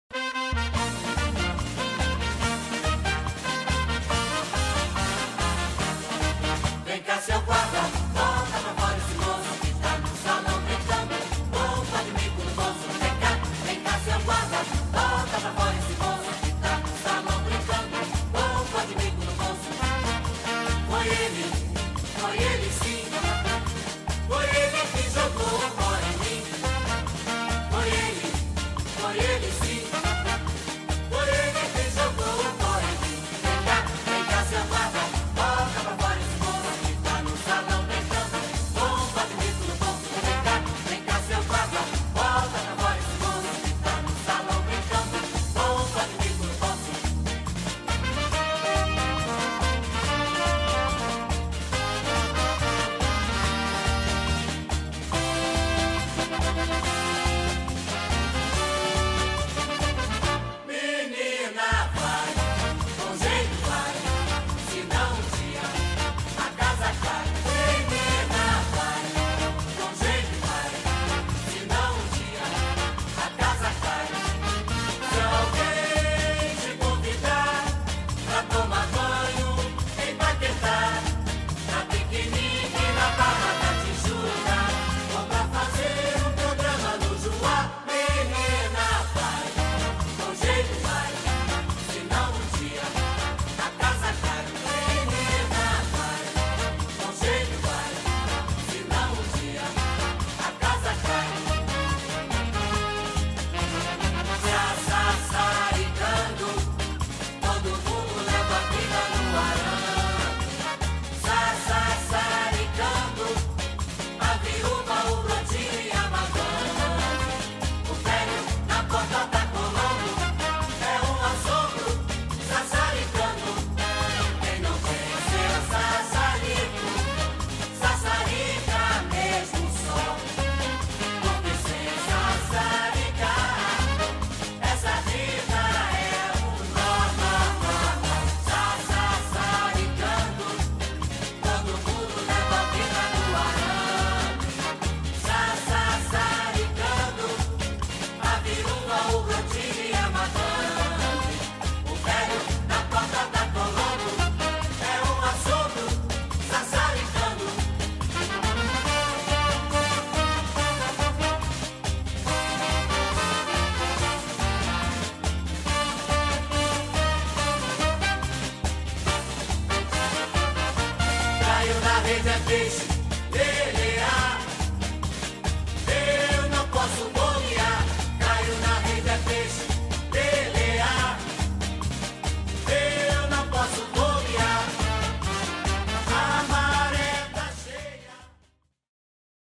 Carnaval 2015 Dr. José Ferreira
O Carnaval de 2015 foi comemorado ao som de marchinhas, foi uma bela festa!